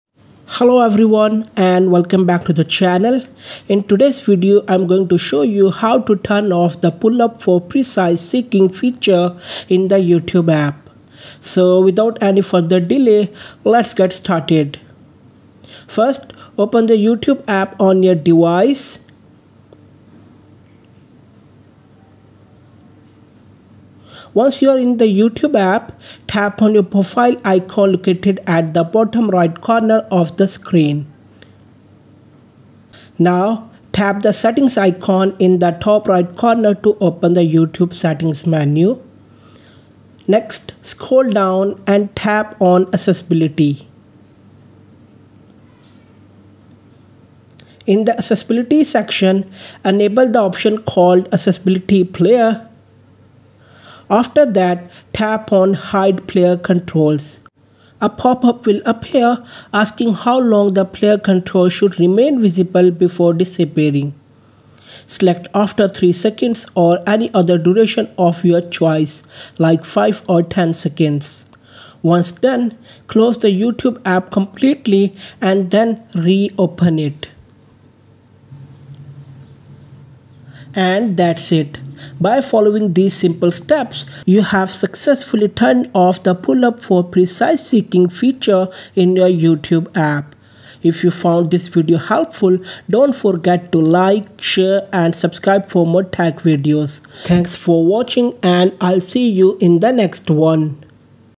How to Solve the Problem in YouTube Pull Up For Precise Seeking
Hello everyone, and welcome back to the channel!
In today’s video, you will learn how to turn off the “Pull up for precise seeking” feature in the YouTube app.
Step 1:
First, open the YouTube app on your device.

Step 2:
Once you’re in the app, tap on your profile icon located at the bottom right corner of the screen.

Step 3:
Now, tap the Settings icon in the top right corner to open the YouTube settings menu.

Step 4:
Scroll down and tap on Accessibility.

Step 5:
In the Accessibility section, enable the option called Accessibility Player.
After that, tap on Hide Player Controls.

Step 6:
A pop-up will appear asking how long the player controls should remain visible before disappearing.
Select “After 3 seconds” or any other duration of your choice, like 5 or 10 seconds.
That's it.